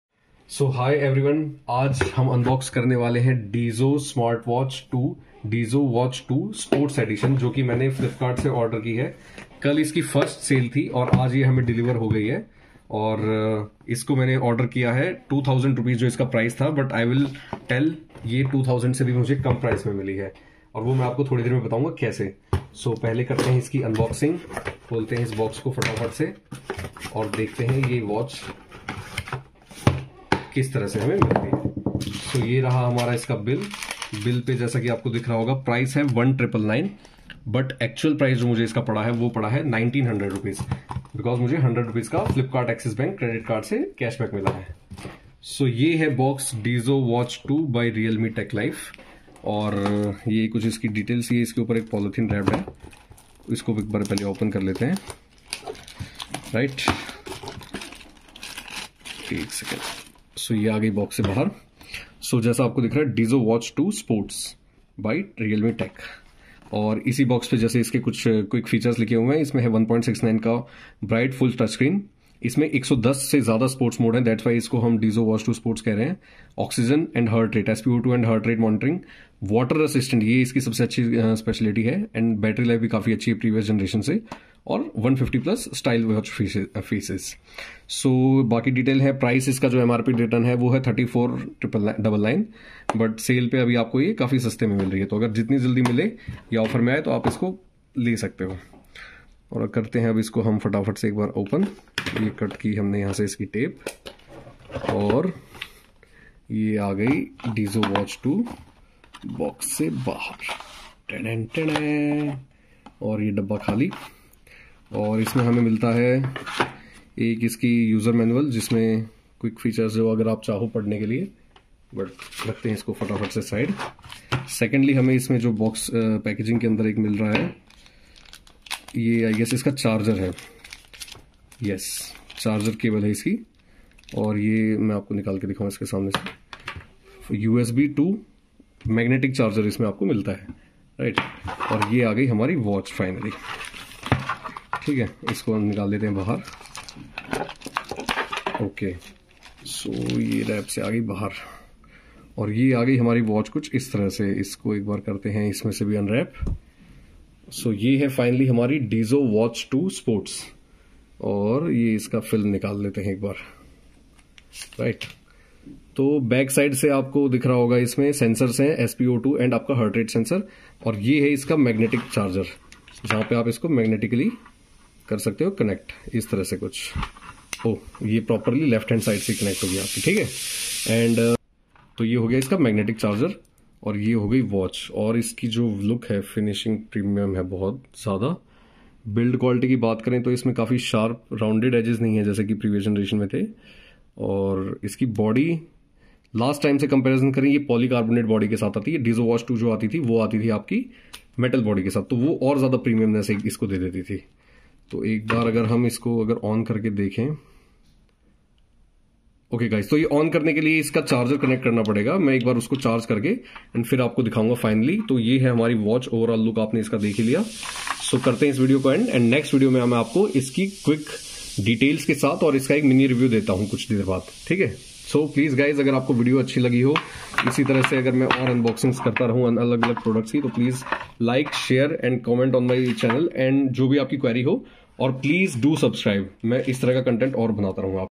🔥DIZO Watch 2 Sports⌚Unboxing and First look @₹1899/- only
hi there, today i will be showing you Unboxing of DIZO Watch 2 Sports Unboxing from Realme.
it's available on Flipkart and i brought this watch in only 1899 using Flipkart axis Bank credit card.